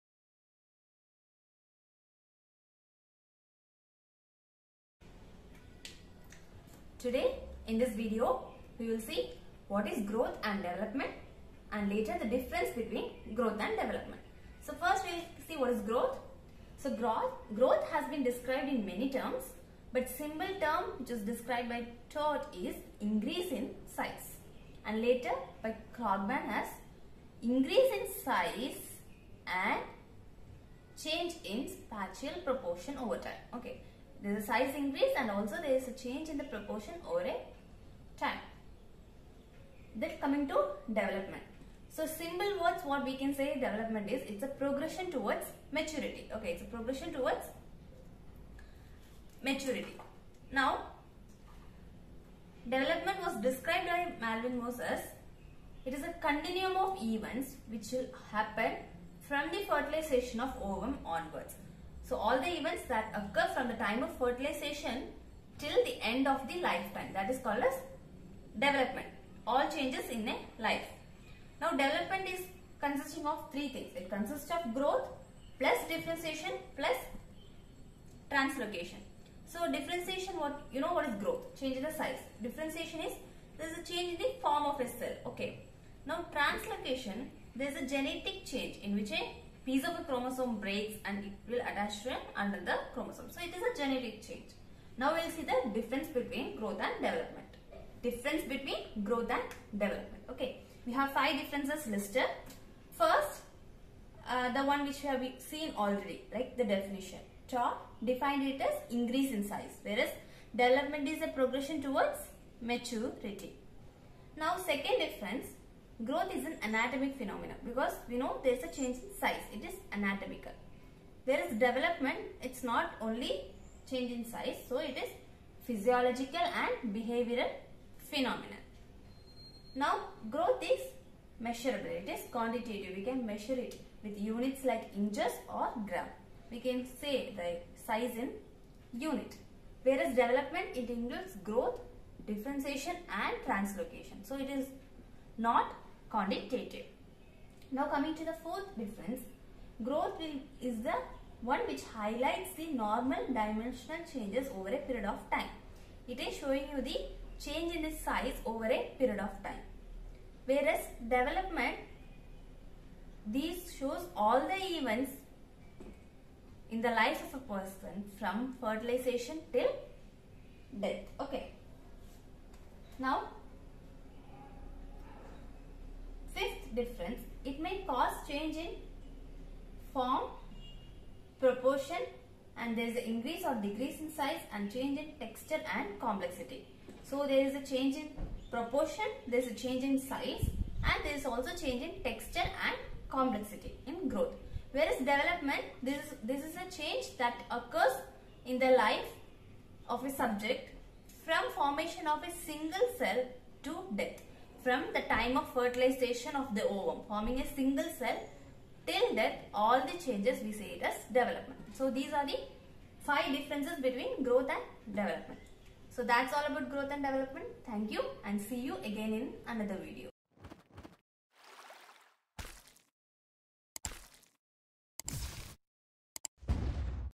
Growth and Development-Orthodontics
In this video I have explained what is growth and development in orthodontics and the difference between growth and development.